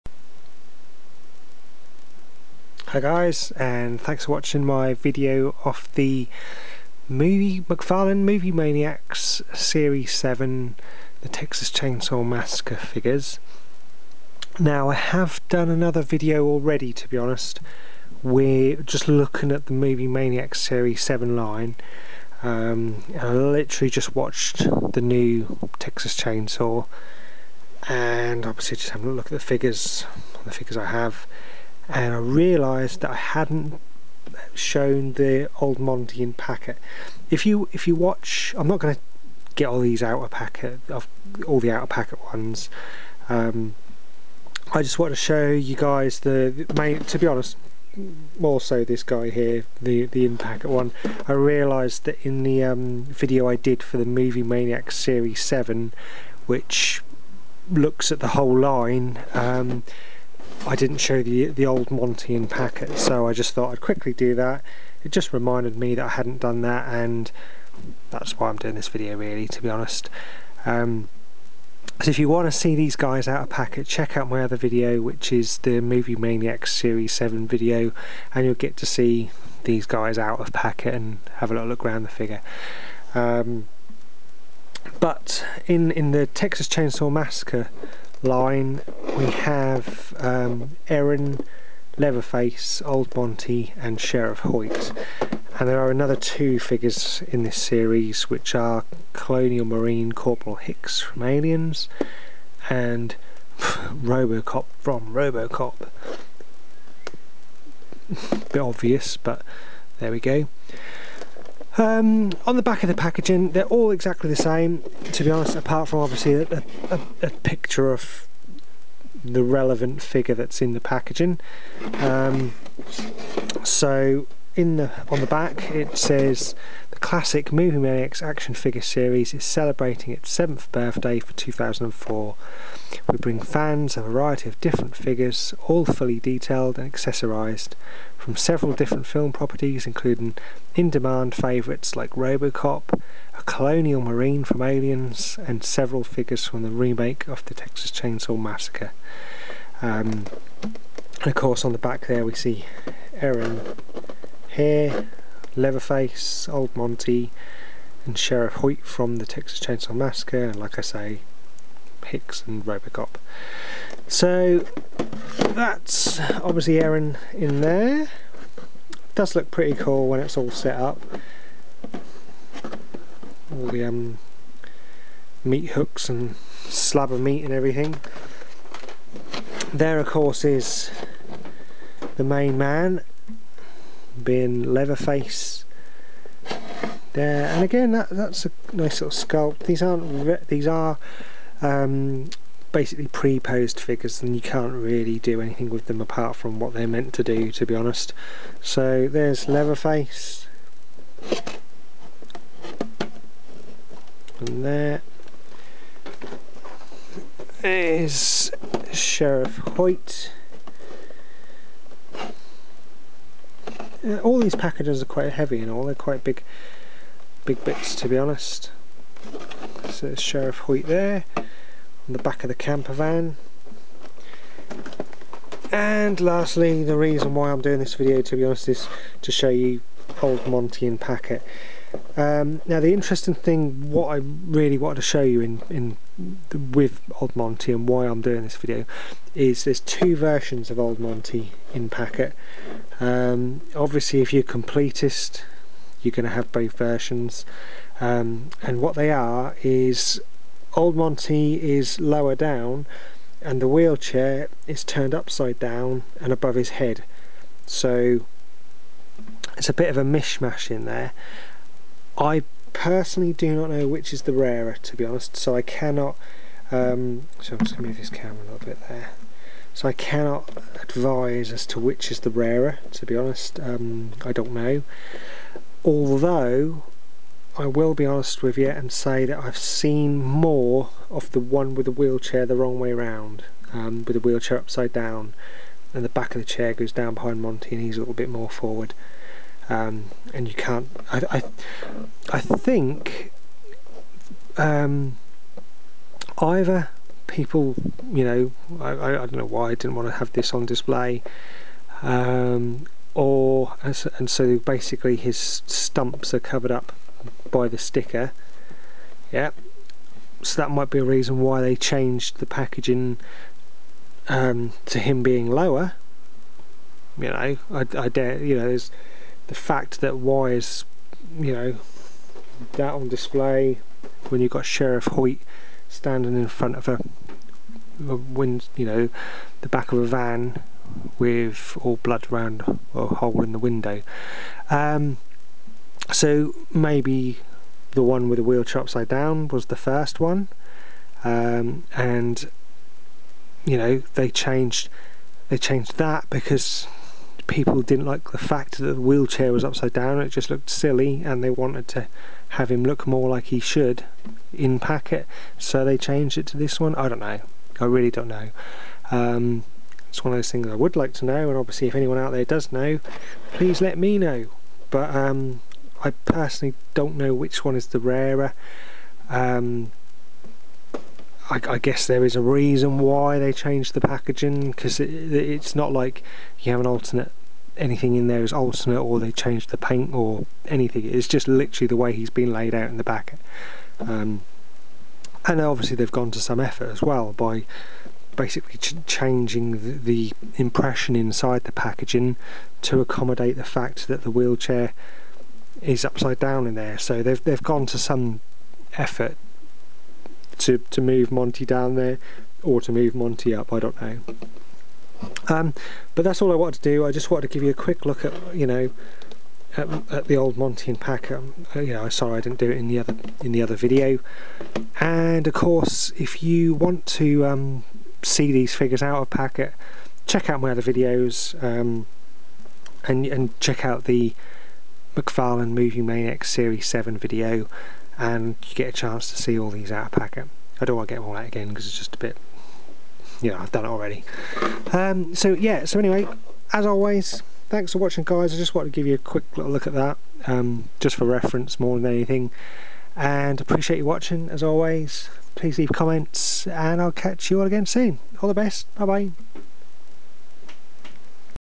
McFarlane Texas Chainsaw Massacre Movie Maniacs Figures
A quick look at the (in packet) McFarlane Movie Maniacs 7 Texas Chainsaw Massacre figures... please check out my Movie Maniacs 7 video for the complete set out of packet.